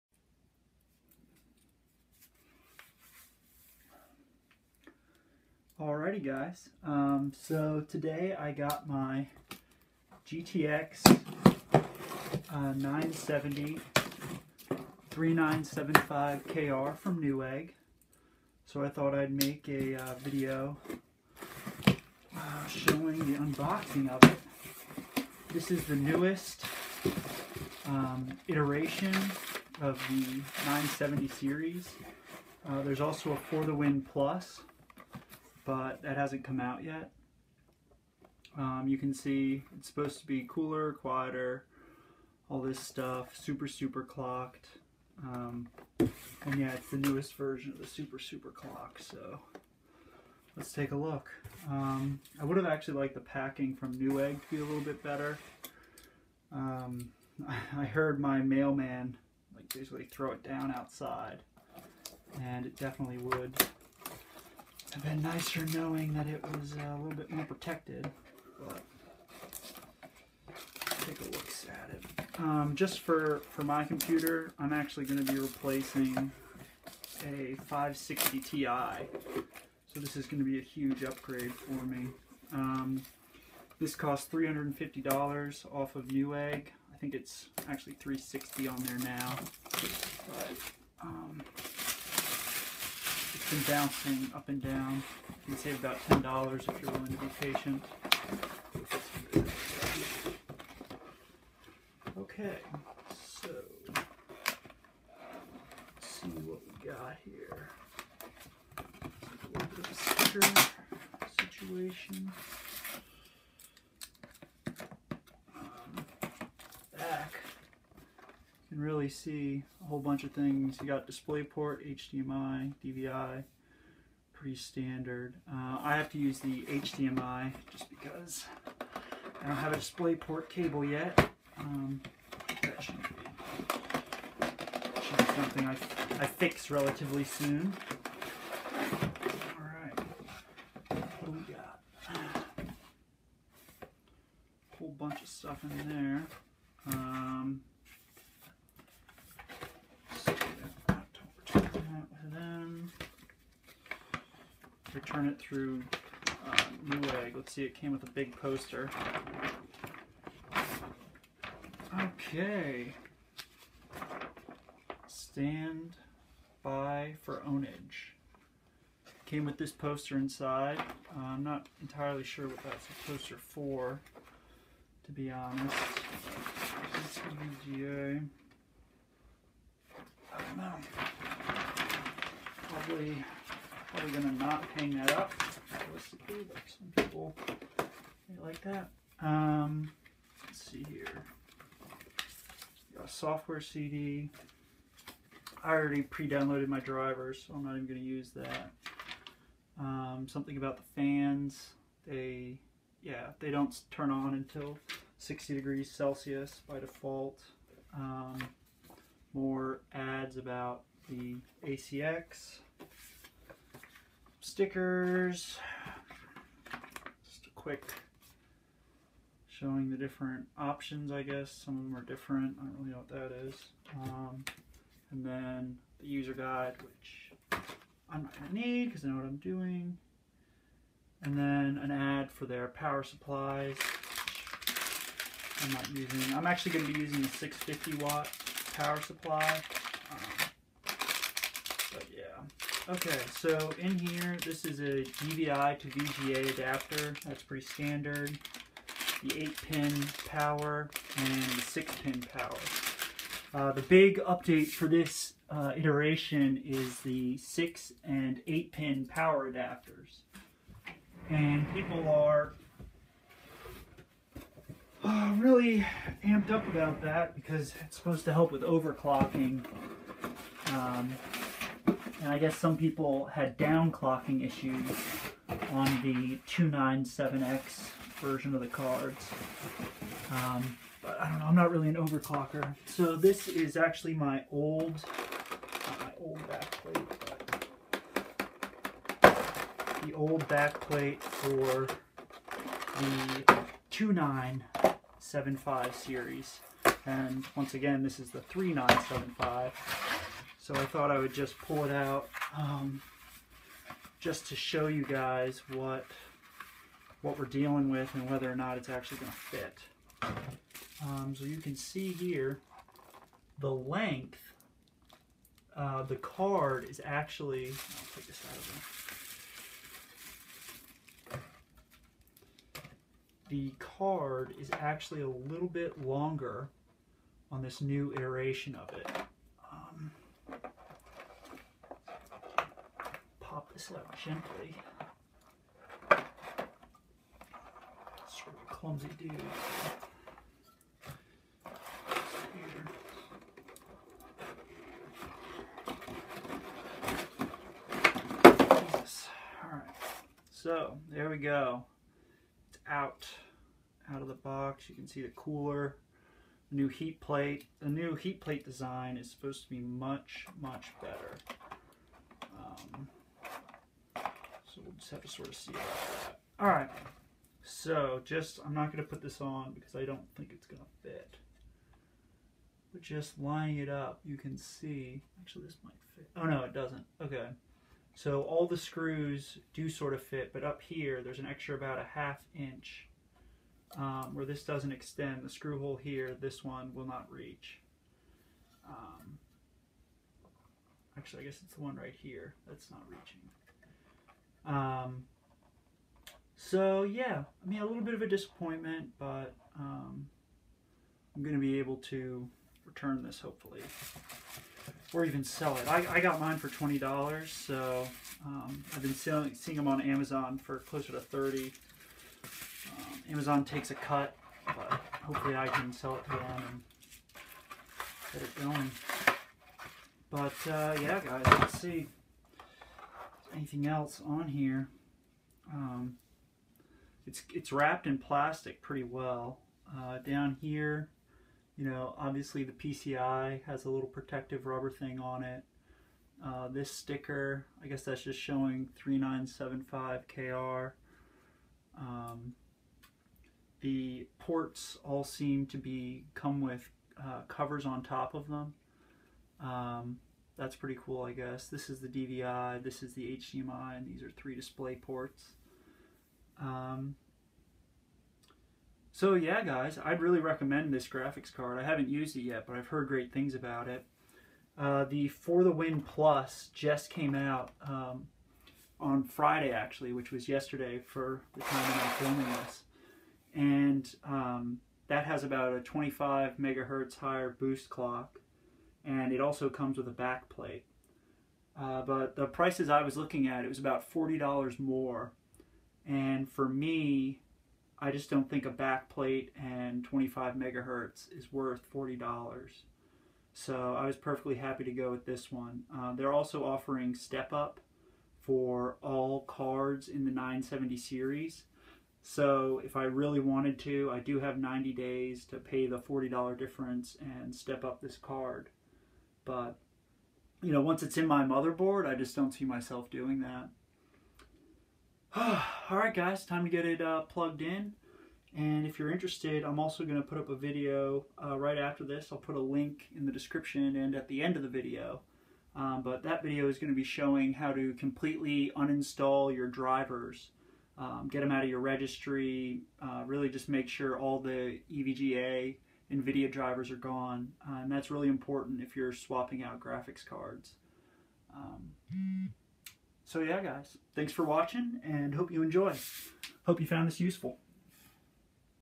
Unboxing EVGA GTX 970 SSC 3975-KR
ASIC 78.3%, boost clock 1429.8MHz, max temp 69C, average 68C

Me opening up a  new EVGA GTX 970 SSC 3975-KR!  This new iteration of the EVGA GTX 970 series comes with a new cooling plate to keep temps real low.  The FTW+ (3978-KR) ships with the new backplate but the SSC (3975-KR does not.  The "old" back plate for the other EVGA GTX 970's do NOT fit on this card!  Skip to 5:00 if you are confused.  I bought mine off of NewEgg for $350. No tax, shipped free.  Mine has no coil whine (yet).  The EVGA GTX970 FTW+ 3978-KR does have a backplate (and slightly higher clocks)